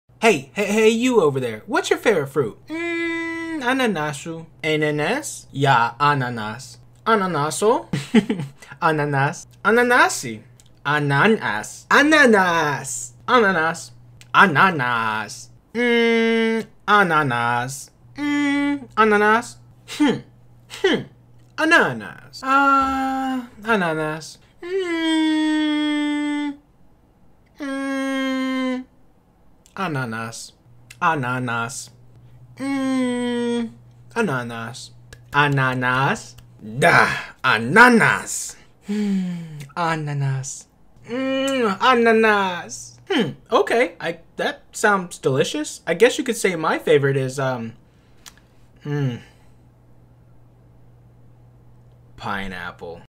PINEAPPLE IN DIFFERENT LANGUAGES
English likes to be completely different than other languages, especially when saying pineapple!